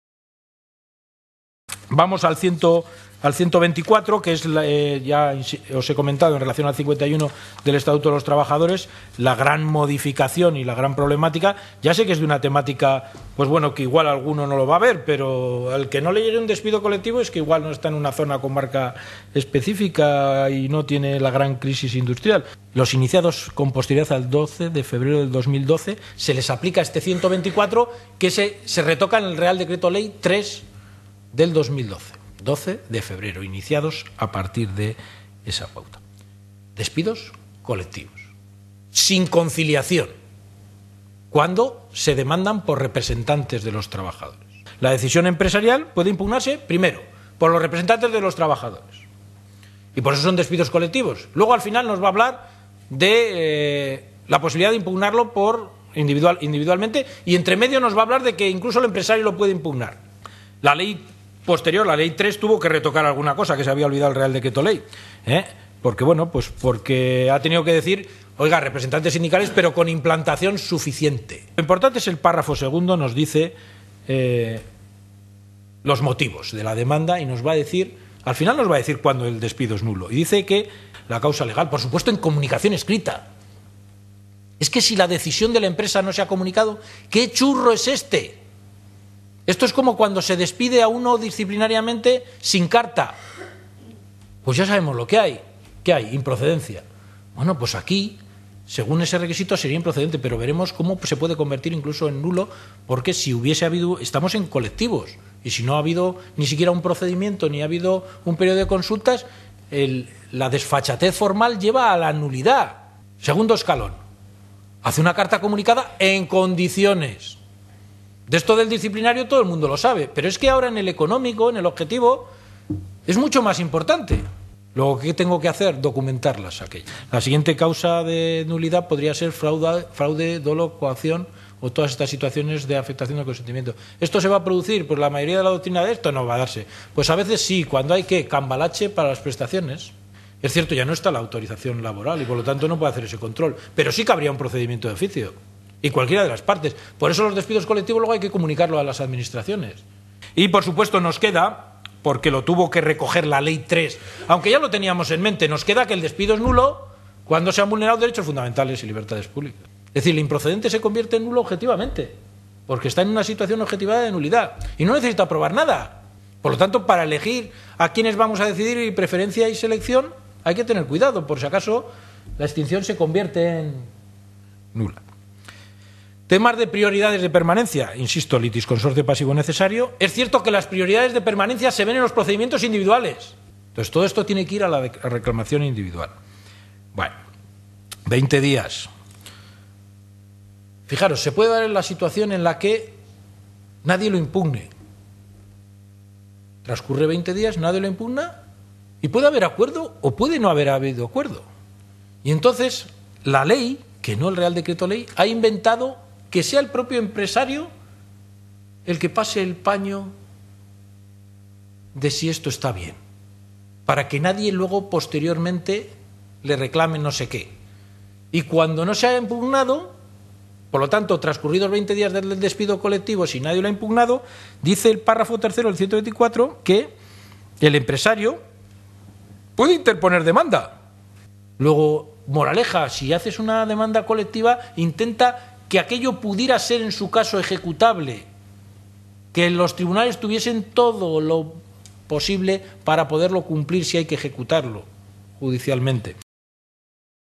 Aspectos procesales de la Reforma Laboral - Juan Carlos Benito-Butrón
VIDEOS CEN 11/04/13
Juan Carlos Benito-Butrón en la jornada "Reforma laboral y aspectos procesales" perteneciente al ciclo "Balance de la reforma laboral de 2012".